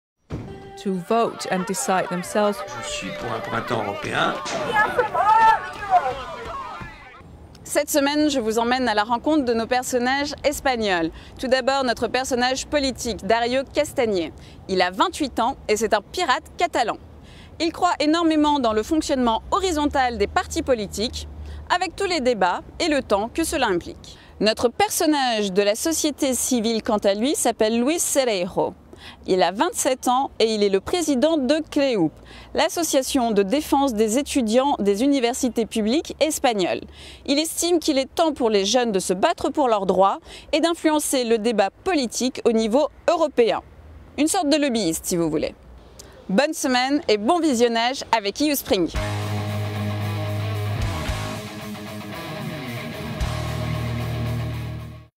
EusprYng Episode 5: Meet our Spanish characters
EU SprYng is an on-line experience on the Y generation facing the 2014 European Parliament elections EP2014, inspired by the Arab spring